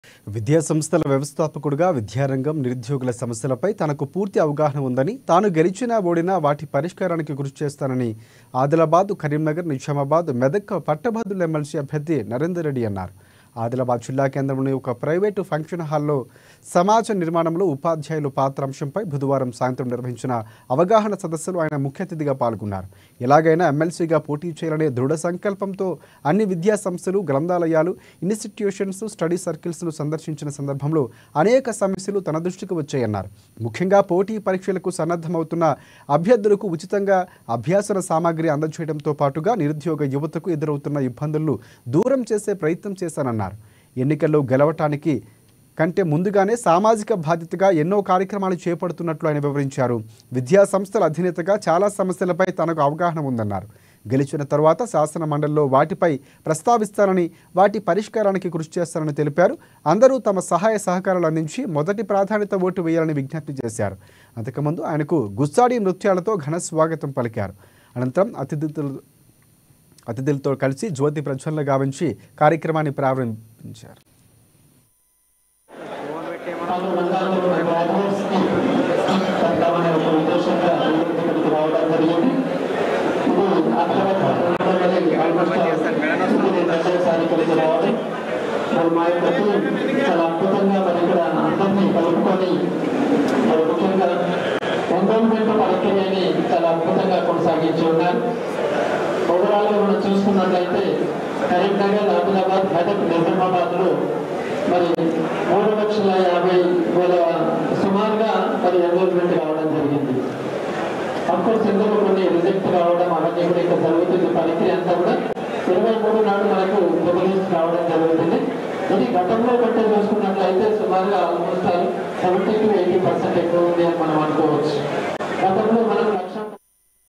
Adilabad : సమాజ నిర్మాణంలో ఉపాధ్యాయుల పాత్రపై అవగాహన సదస్సు నిర్వహణ | Ann Telugu
ANN Telugu LIVE🔴: Telugu News Live | ANN Telugu  24x7 Live Streaming